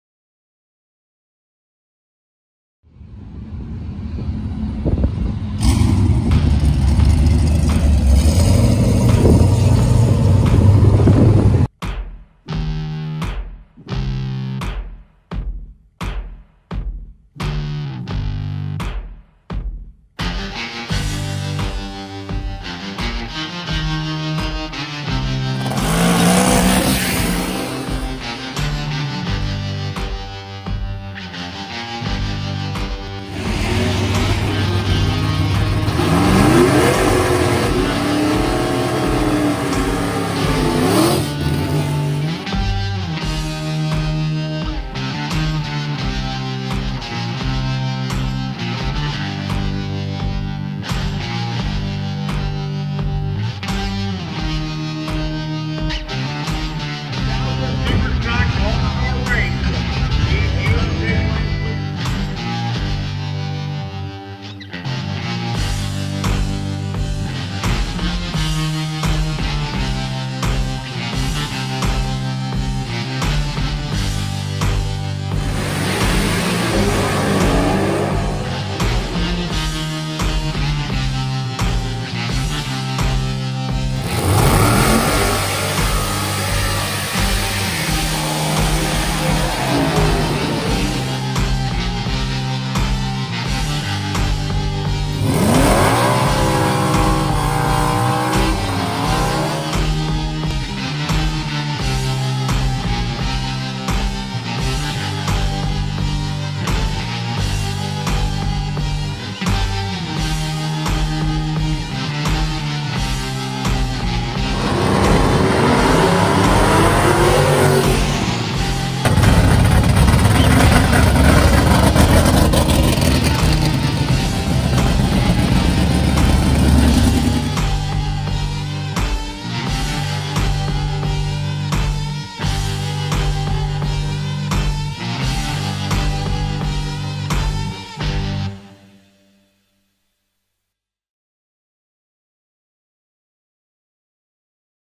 2020 NMCA World Finals Recap
The 2020 NMCA World Street Finals were held at Lucas Oil Raceway on September 24-27.